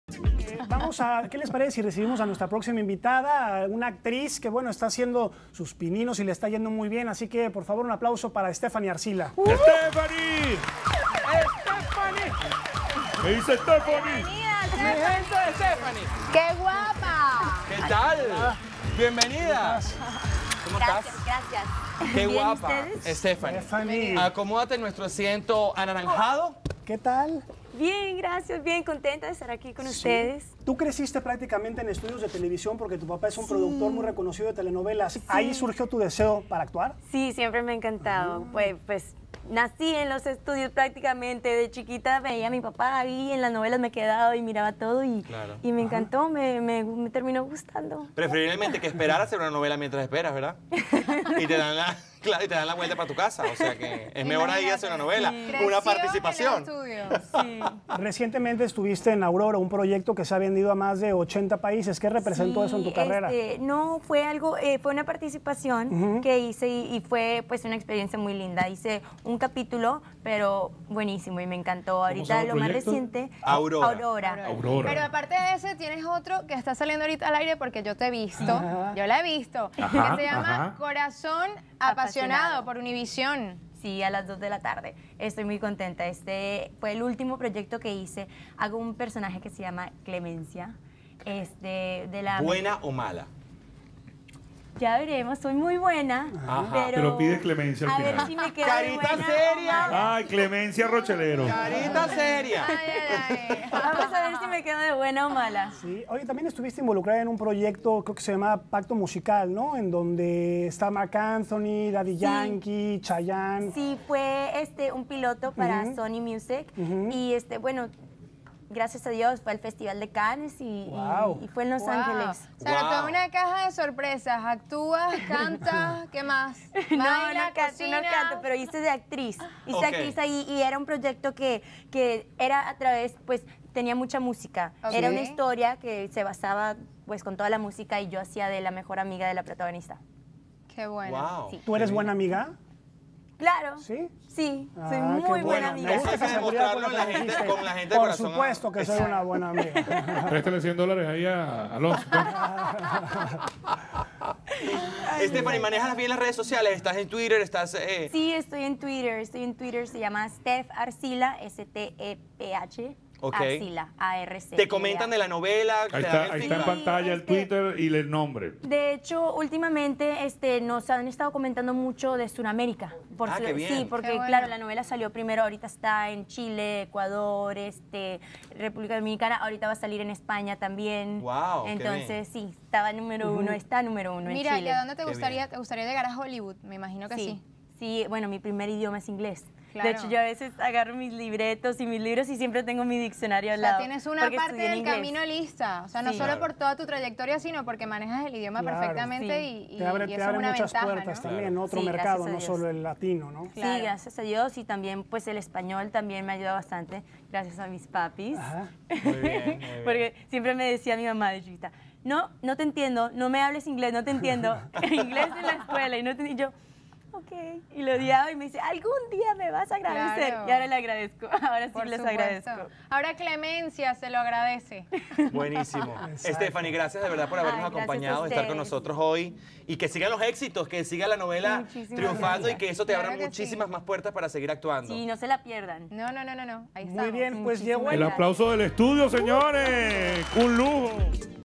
Stephany Arcila en 99TV
La actriz Stephany Arcila que nos cuenta sobre su participación en la novela Aurora, su actuación como Clemencia en su nuevo proyecto Corazón Apasionado. Además de su proyecto musical junto a Marc Anthony, Chayanne y Daddy Yankee llamado pacto musical